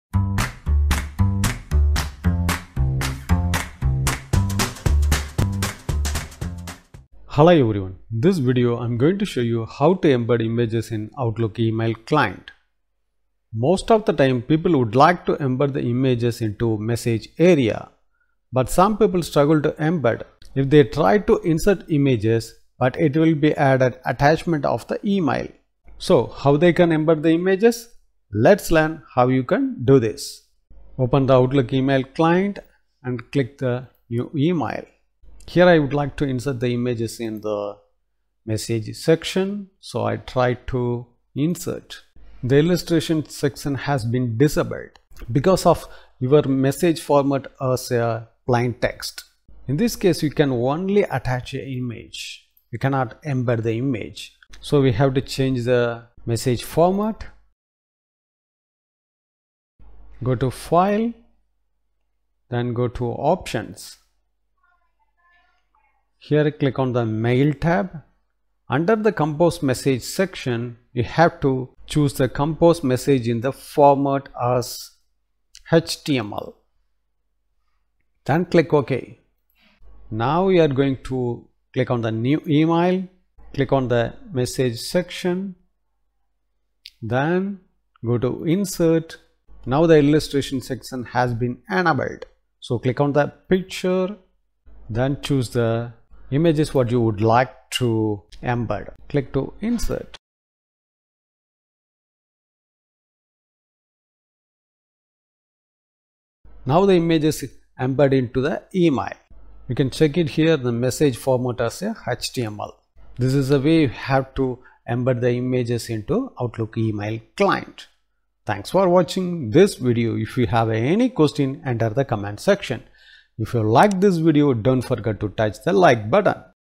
How to Embed Images in Email message on Outlook Email Client
Most of the time people would like to embed the images into the message area. But some people struggle to embed it. if they try to insert images, but it will add an attachment to the email.

This video clearly explains how you can embed the images in the Microsoft Outlook email client.